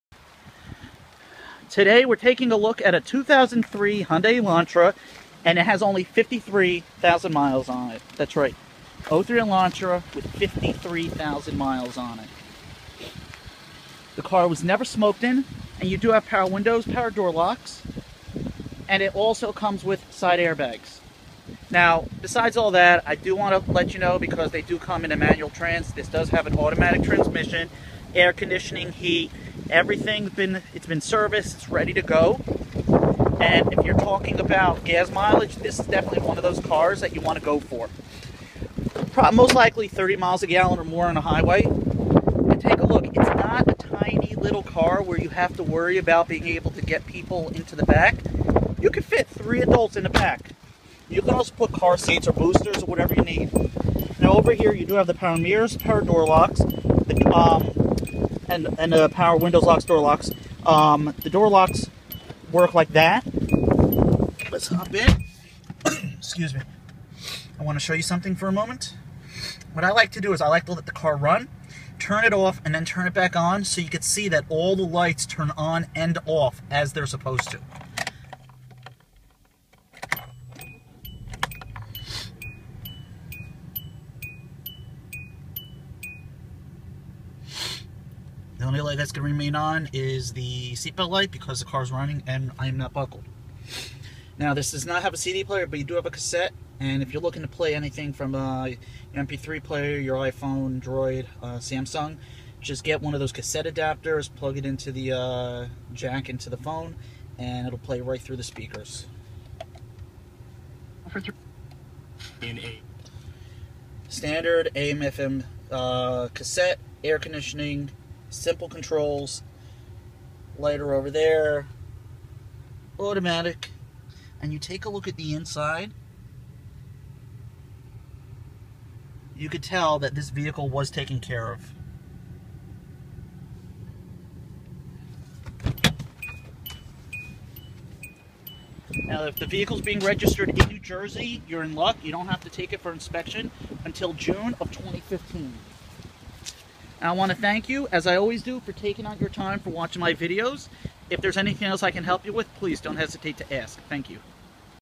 2003 Hyundai Elantra GLS in Edison,NJ
This is a 2003 Hyundai Elantra GLS with 53,000 low miles on it. This low mileage Elantra was never smoked in and comes equipped with plower windows, power door locks, side airbags and a Hyundai remote keyless entry. Prior to leaving our dealership this vehicle is serviced and it comes with a warranty.